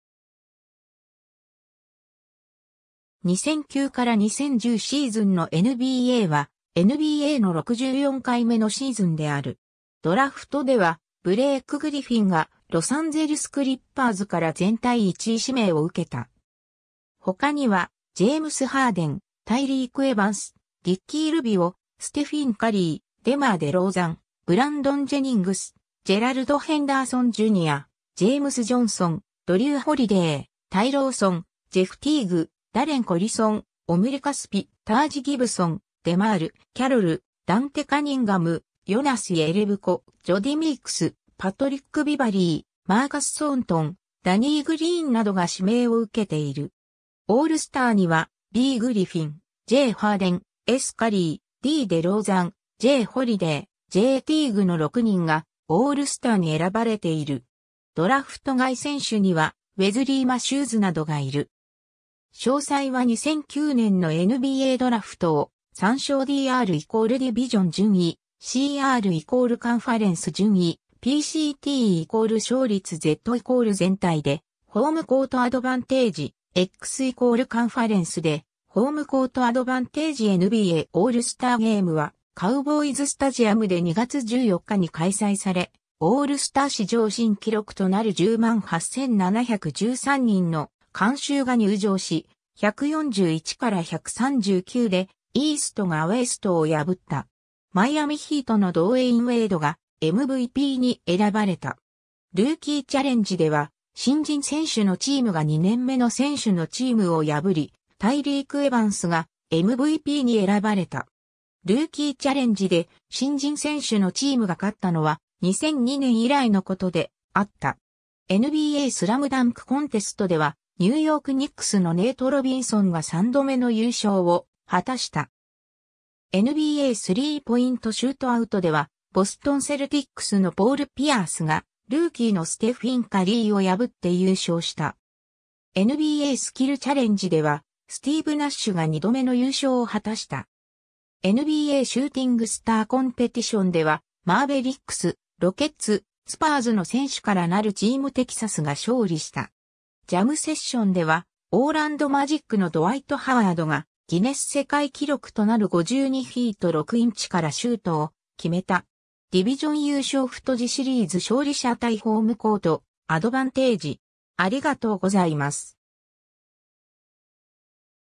2009-2010シーズンのNBA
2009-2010シーズンのNBAは、NBAの64回目のシーズンである。
 ドラフトではブレイク・グリフィンがロサンゼルス・クリッパーズから全体1位指名を受けた。
他にはジェームス・ハーデン、タイリーク・エバンス、リッキー・ルビオ、ステフィン・カリー、デマー・デローザン、ブランドン・ジェニングス、ジェラルド・ヘンダーソン・ジュニア、ジェームス・ジョンソン、ドリュー・ホリデー、タイ・ローソン、ジェフ・ティーグ、ダレン・コリソン、オムリ・カスピ、タージ・ギブソン、デマール・キャロル、ダンテ・カニンガム、ヨナス・イェレブコ、ジョディ・ミークス、パトリック・ビバリー、マーカス・ソーントン、ダニー・グリーンなどが指名を受けている。
 オールスターにはB・グリフィン、J・ハーデン、S・カリー、D・デローザン、J・ホリデー、J・ティーグの6人がオールスターに選ばれている。
 ドラフト外選手にはウェズリー・マシューズなどがいる。
 詳細は2009年のNBAドラフトを参照 DR = ディビジョン順位, CR = カンファレンス順位,PCT = 勝率 Z = 全体でホームコートアドバンテージ,ｘ = カンファレンスでホームコートアドバンテージ NBAオールスターゲームは、カウボーイズ・スタジアムで2月14日に開催され、オールスター史上新記録となる108,713人の観衆が入場し、141-139でイーストがウェストを破った。
マイアミ・ヒートのドウェイン・ウェイドがMVPに選ばれた。
ルーキー・チャレンジでは新人選手のチームが2年目の選手のチームを破り、タイリーク・エバンスがMVPに選ばれた。
ルーキーチャレンジで新人選手のチームが勝ったのは、2002年以来のことであった。
NBAスラムダンクコンテストではニューヨーク・ニックスのネイト・ロビンソンが3度目の優勝を果たした。
NBAスリーポイント・シュートアウトではボストン・セルティックスのポール・ピアースがルーキーのステフィン・カリーを破って優勝した。
NBAスキルチャレンジではスティーブ・ナッシュが2度目の優勝を果たした。
NBAシューティングスターコンペティションではマーベリックス、ロケッツ、スパーズの選手からなるチームテキサスが勝利した。
ジャムセッションでは、オーランド・マジックのドワイト・ハワードがギネス世界記録となる52フィート6インチからシュートを決めた。
 *ディビジョン優勝 太字 シリーズ勝利 斜体 ホームコート・アドバンテージ